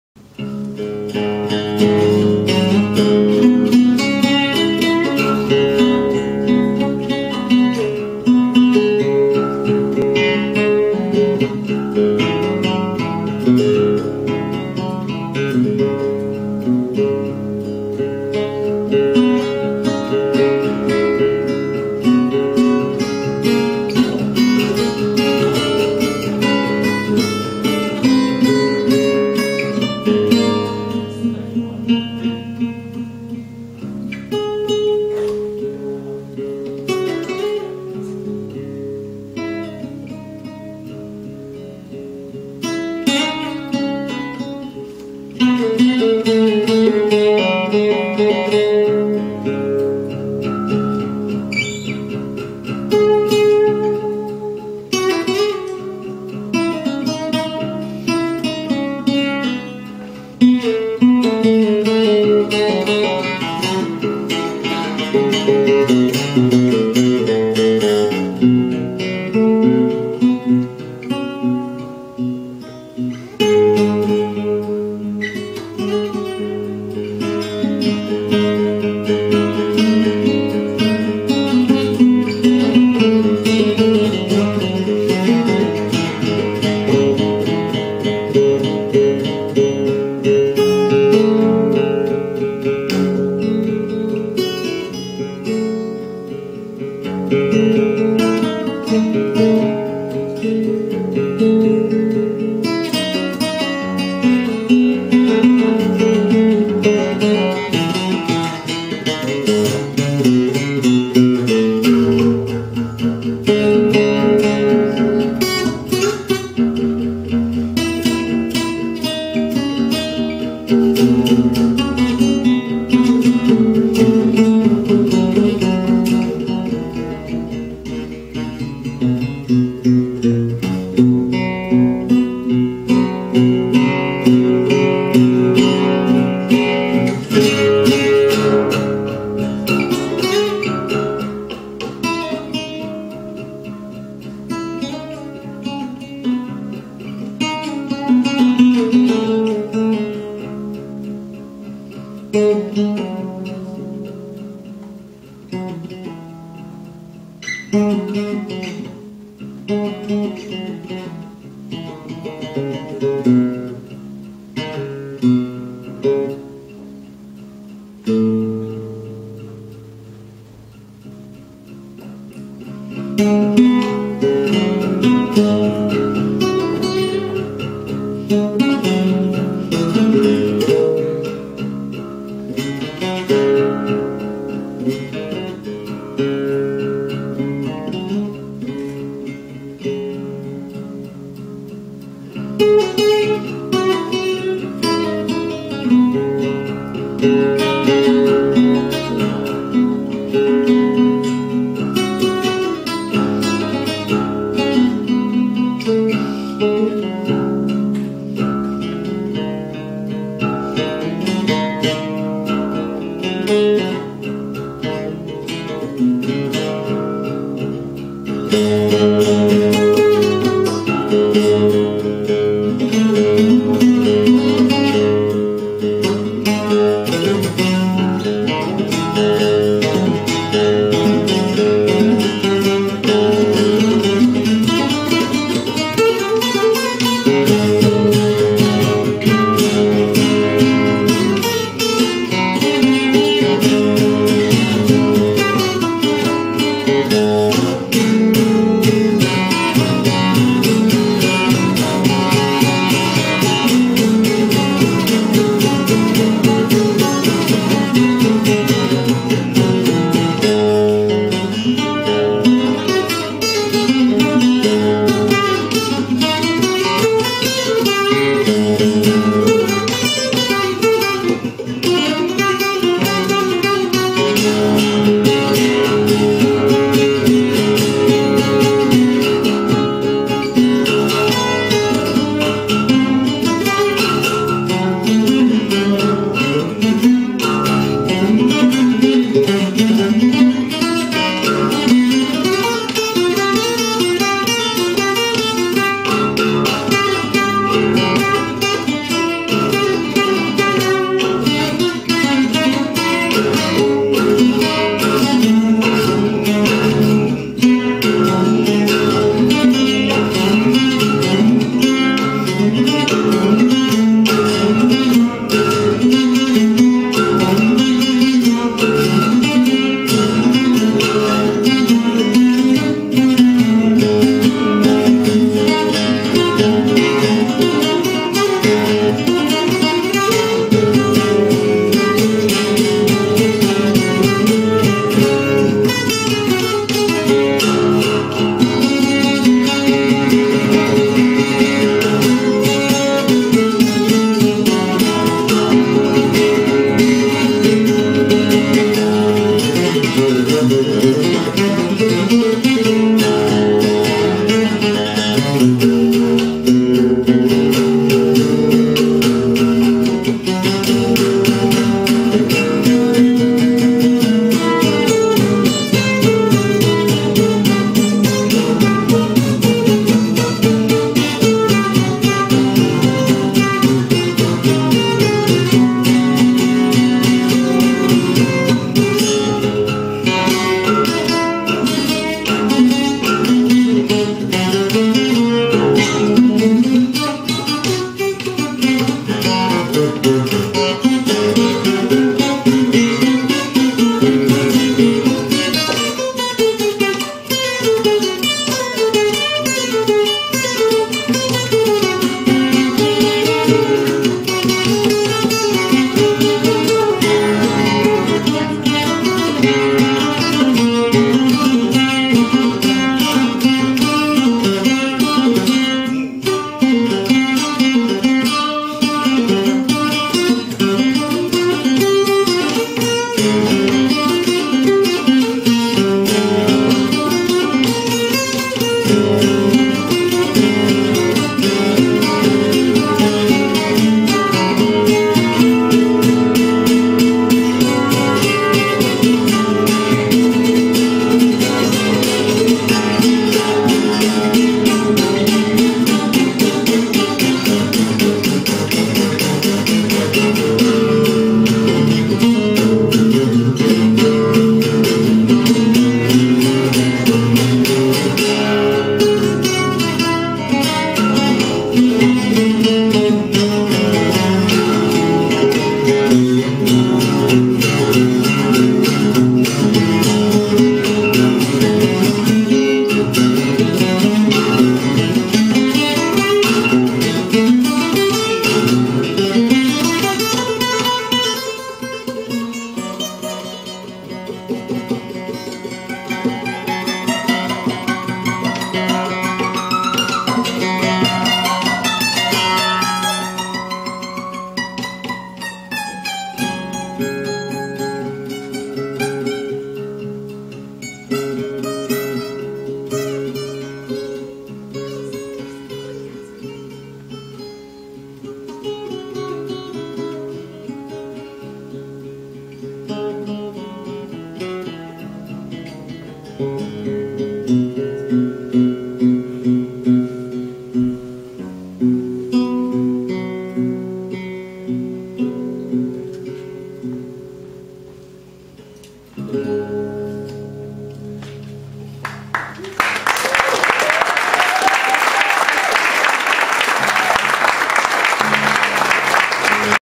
Marc Ribot at The Kessler Theater in Dallas
Recorded live on December 1, 2011 at the historic Kessler Theater in X+/North Oak Cliff.